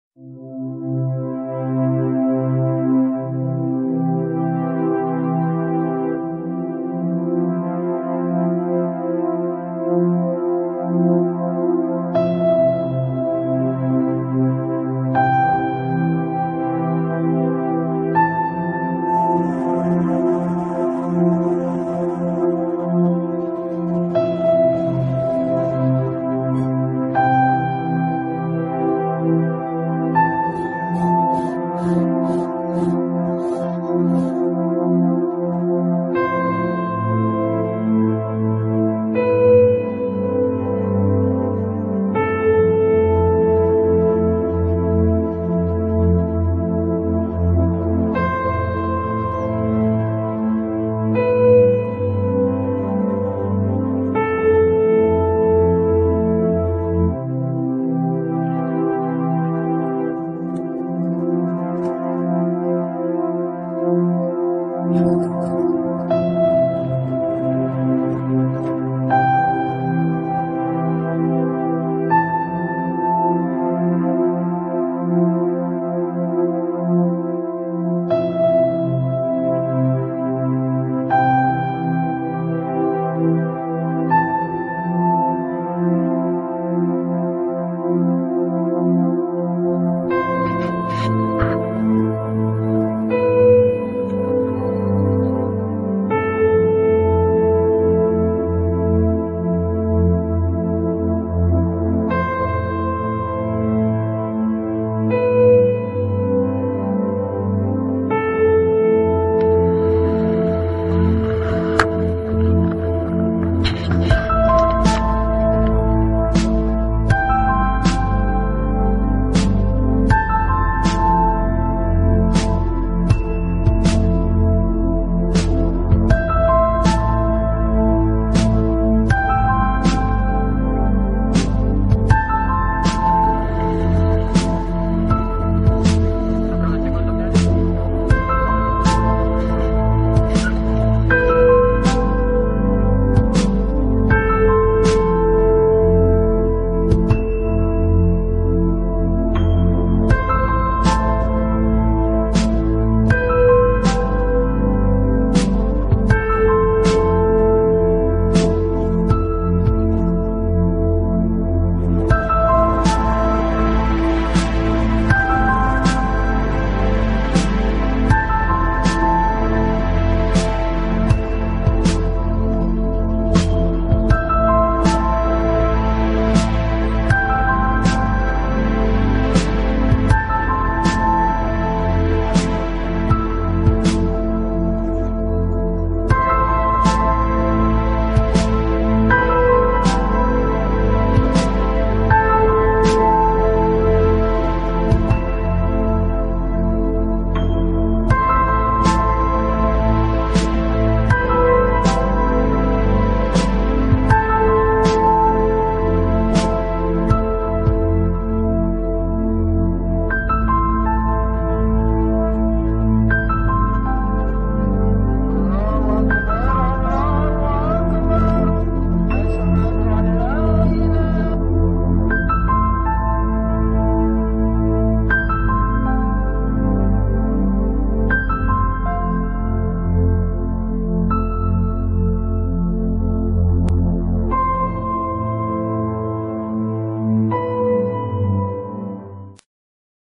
Susur Sungai Karang Mumus Part 3
Susur Sungai Karang Mumus ini dilakukan oleh IMAPA Unmul dalam rangka peringatan Hari AIr 2016.